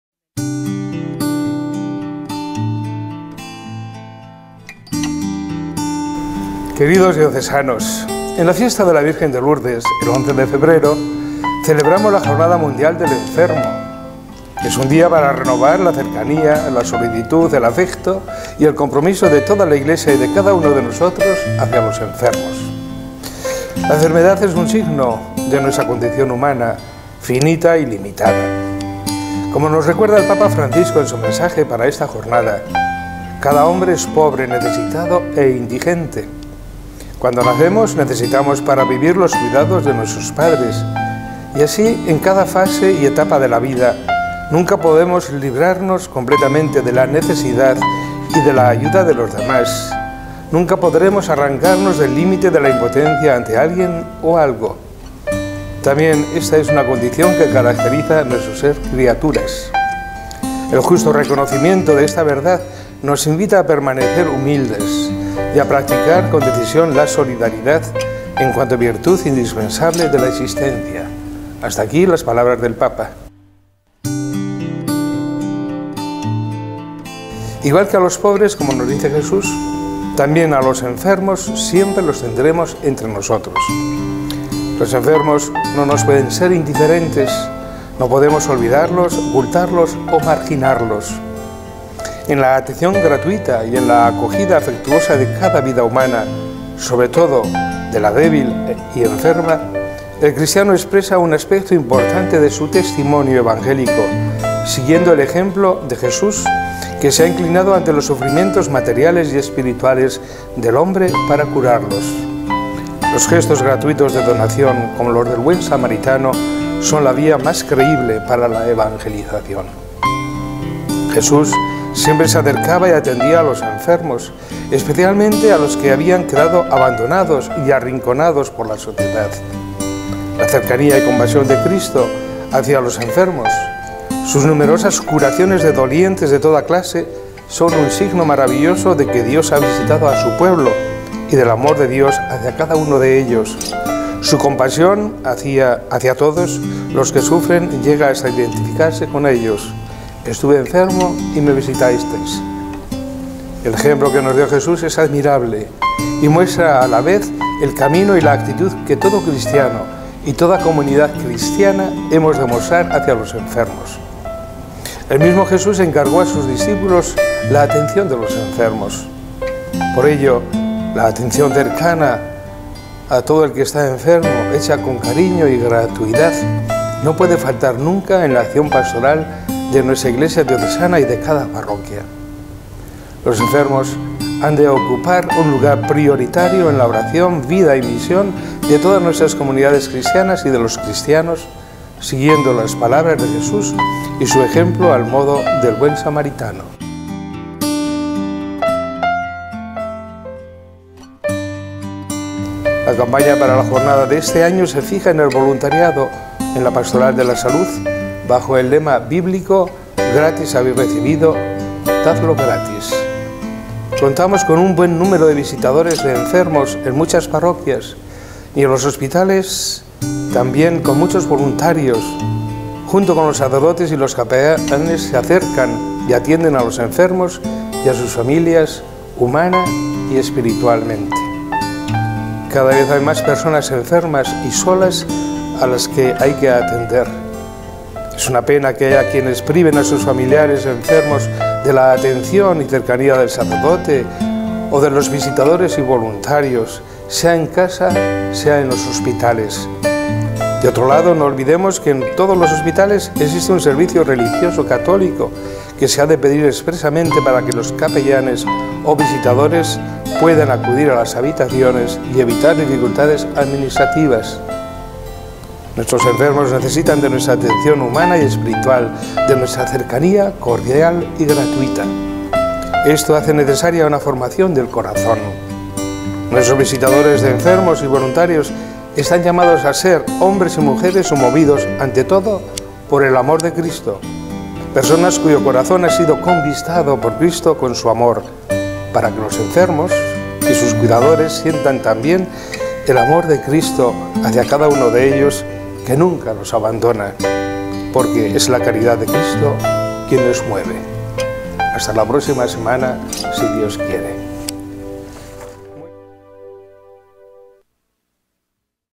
Amor gratuito y cercano a los enfermos
Mons. Casimiro López Llorente dedica su carta del domingo 10 de febrero a la Jornada Mundial de los Enfermos.